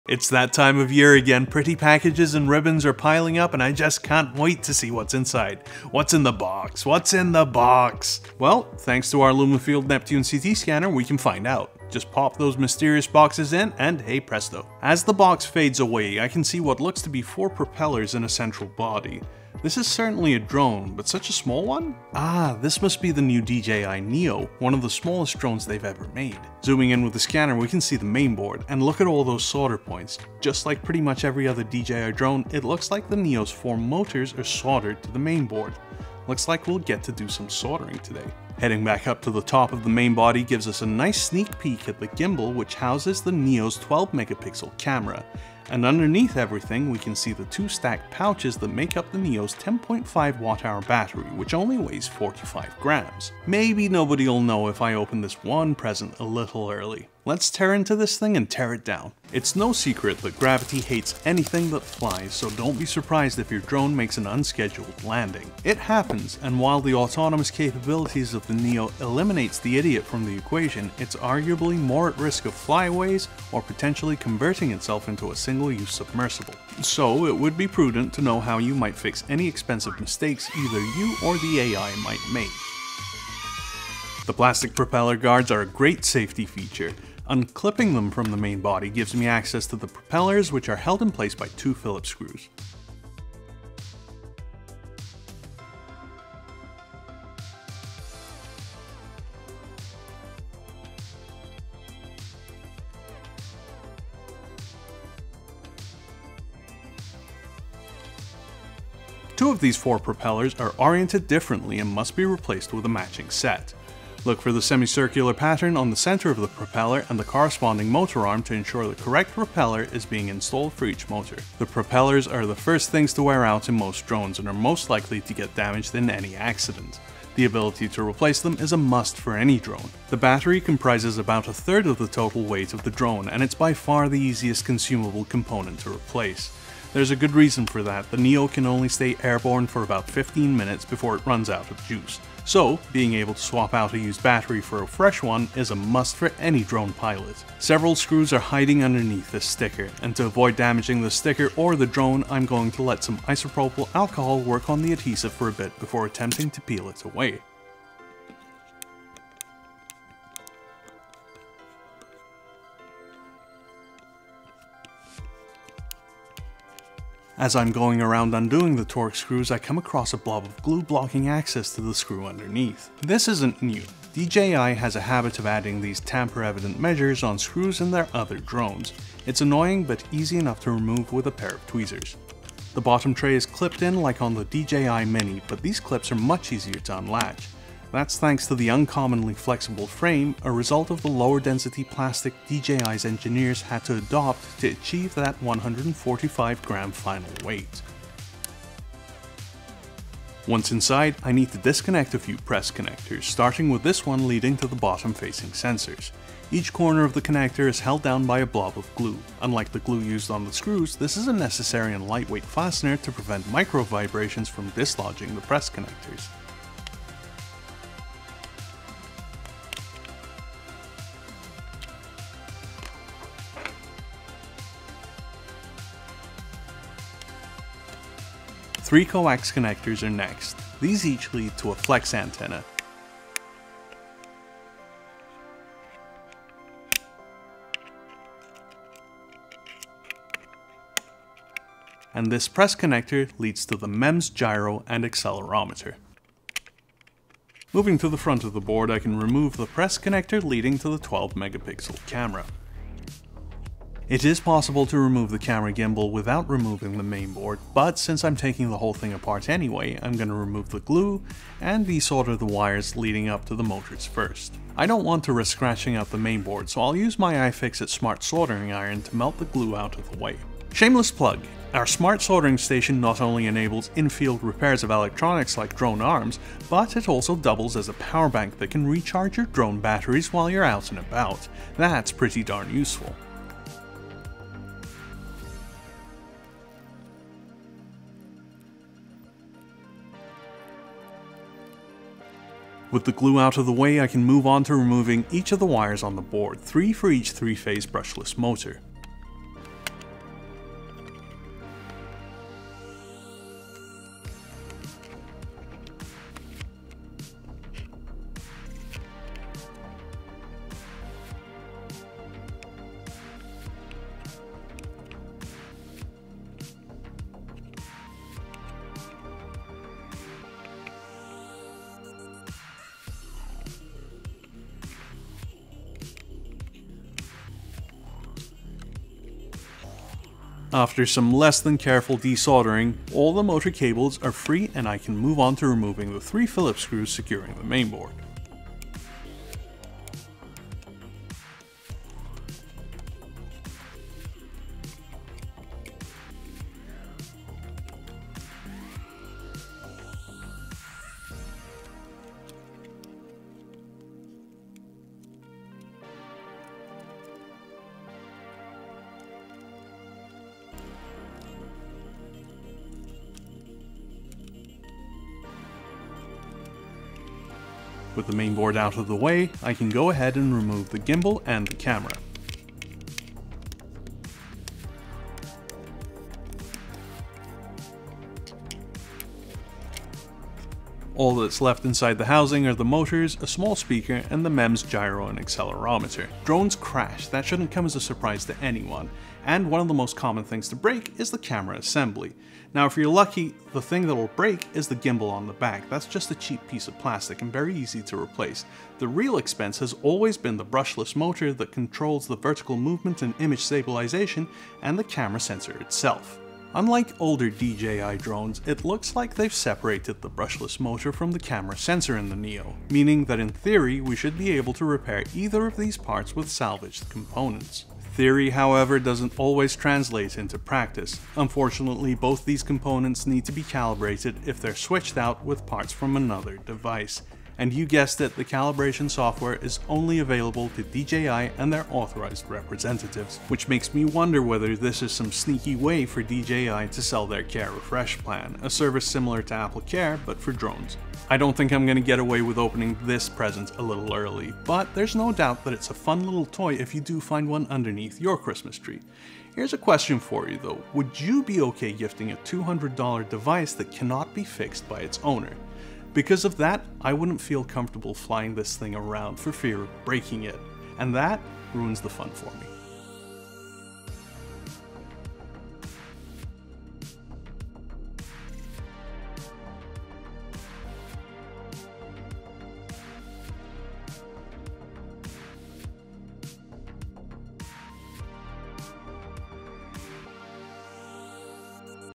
WHAT'S IN THE BOX? DJI Neo Drone Scanned, Torn Down, and Exposed!
It’s that time of year again. Pretty packages and ribbons are piling up and I just can’t wait to see what’s inside. What’s in the box….What’s in the box? Ah! This MUST be the new DJI Neo, one of the smallest drones DJI has ever made. Maybe nobody’ll know if I open this one present a little early. Let’s tear into this thing and tear it down!

This teardown is not a repair guide. To repair your DJI Neo, use the service manual at iFixit.com.

Chapters
00:00 The DJI Neo Teardown
00:12 CT Scans of the DJI Neo
00:59 DJI Neo Teardown Begins!
01:25 Removing the DJI Neo’s Propellers
02:16 Removing the DJI Neo’s Battery
02:29 Unscrewing the DJI Neo’s Bottom Tray
03:23 Freeing the DJI Neo’s  Mainboard
04:20 Desoldering the DJI Neo’s Motor Cables
05:39 Removing the DJI Neo’s Mainboard
06:10 Removing the DJI Neo’s Camera Assembly
06:26 DJI Neo’s Repairability